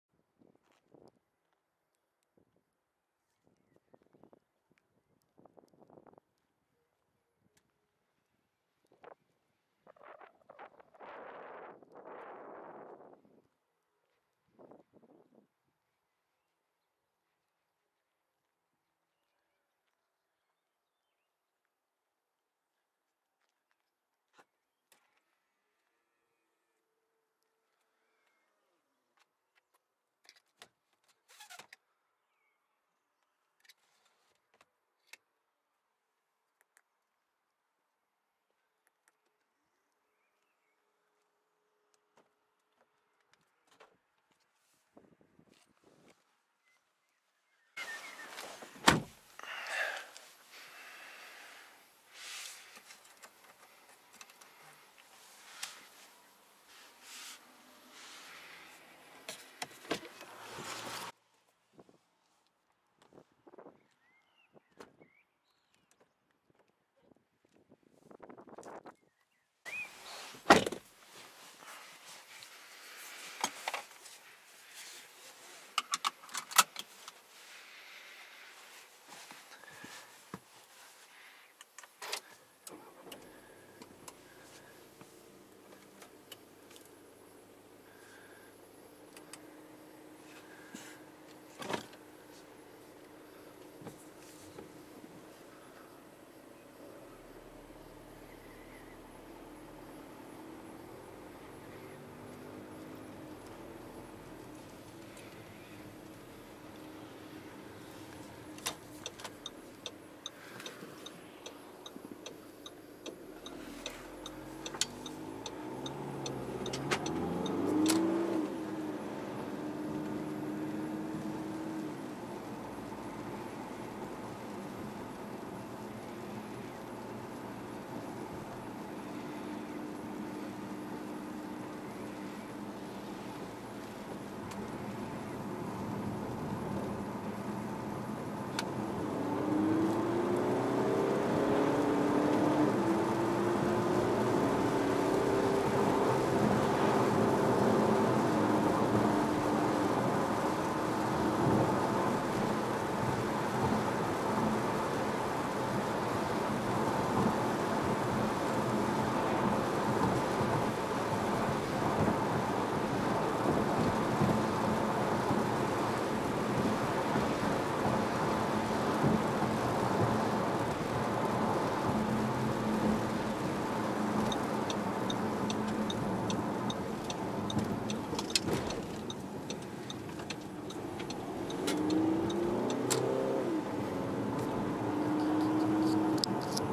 New Audi Q3 Sport 2.0 TDi 135 kW, 2017: 4K POV static and drive
Natočeno akční kamerou Sony Action Cam FDR-X3000.